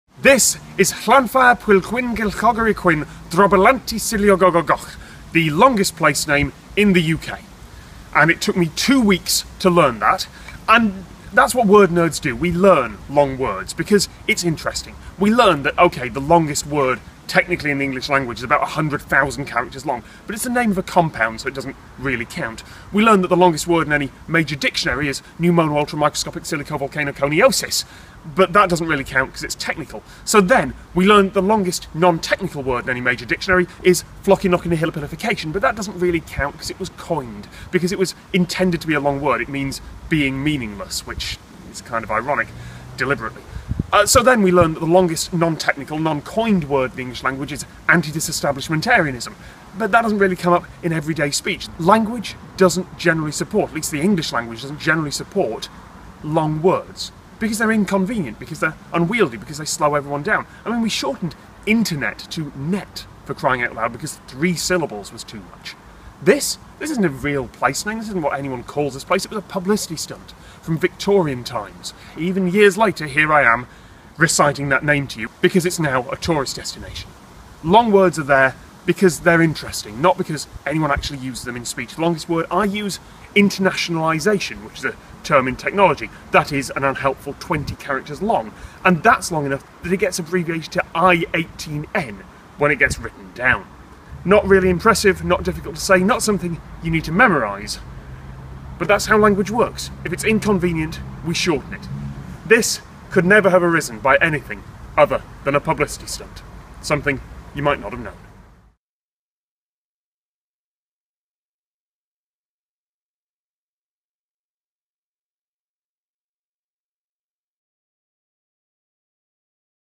What's The Longest Word In The English Language?
- @tomscott - I'm getting a bit linguistic in this week's video, from the Welsh village of Llanfair­pwllgwyn­gyllgo­gery­chwyrn­drobwll­llanty­silio­gogo­goch. And as often happens with linguistics, the answer depends on how you define things. What counts as a word, after all?